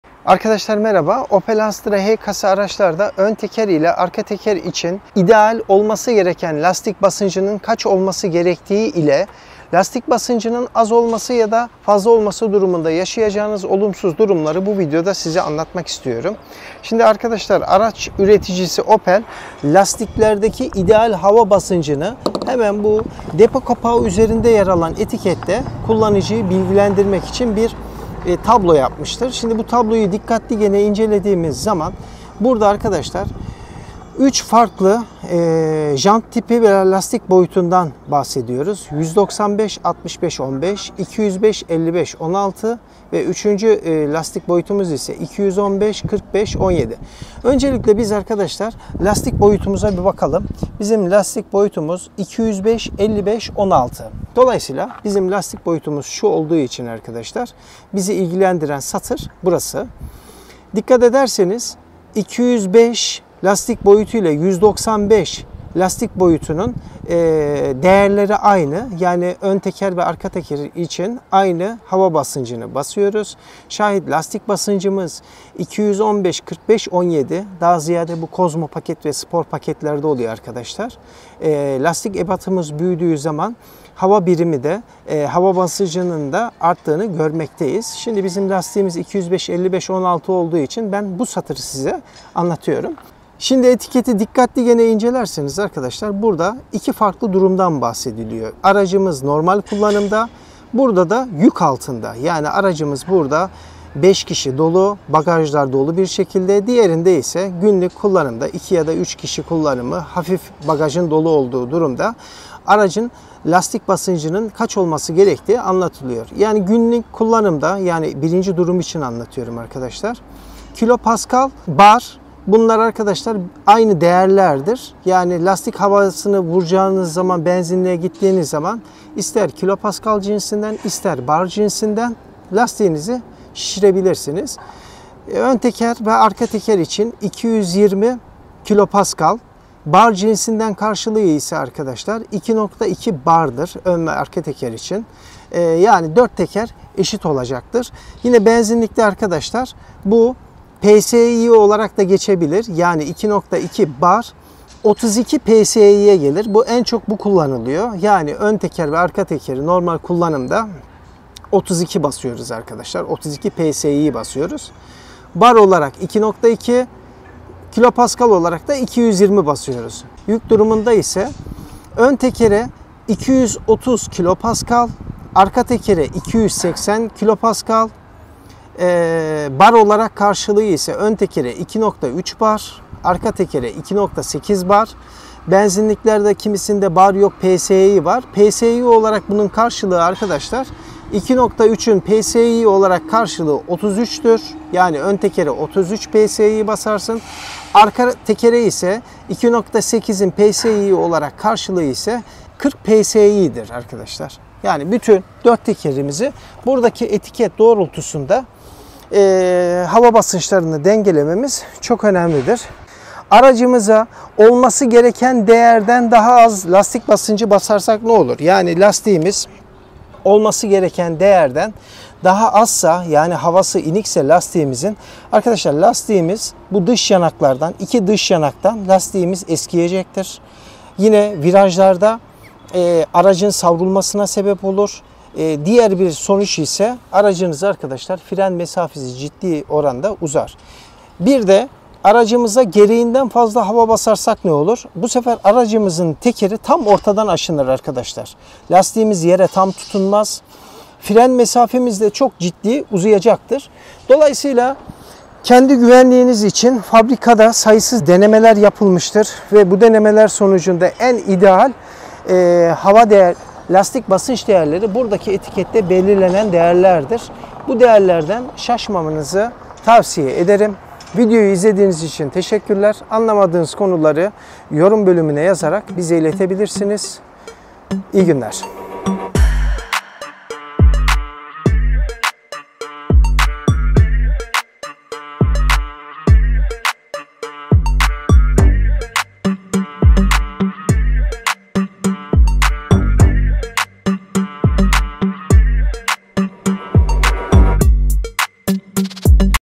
Lastik Basıncı Kaç Olmalı? Opel Astra H
Opel astra h kasa araçlarda lastik basınçlarının kaç kpa br psi olduğunu depo kapağındaki tabloyu inceleyerek öğrenebilirsiniz.

Lastikleri doğru hava basıncı ile kullanmak sürüş güvenliği ve konforu açısından çok önemlidir. Sizde videodaki işlem adımlarını takip ederek opel astra h kasa aracınız lastik basınçlarını doğru bir şekilde öğrenip lastiklerinizi bu şekilde kullanabilirsiniz.

Lastik basıncı az olursa; Virajlarda savrulma, fren mesafesinde uzama ve lastik yanaklarında aşınma meydana gelir.
Lastik basıncı çok olursa; Lastik orta bölgeden aşınır, araç konforsuz hale gelir ve fren mesafesi uzar.

Bu sebeple aracınızı fabrikanın uzun denemeler sonucunda belirlemiş olduğu değerde kullanınız.